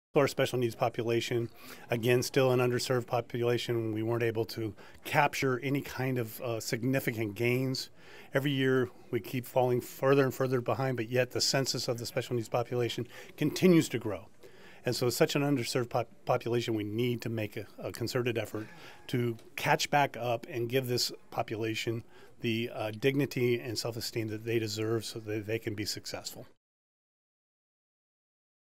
Assemblymember Frazier: We Must Provide for Californians with Special Needs
(Sacramento) - Assemblymember Jim Frazier (D-Discovery Bay), chair of the Assembly Select Committee on Intellectual and Developmental Disabilities, criticized the State Budget adopted by the Legislature for not doing enough for Californians with special needs. “The census of the special needs population continues to grow,” Assemblymember Frazier said. “With such an underserved population we need to make a concerted effort to catch back up and give this population the dignity and self-esteem they deserve to be successful.”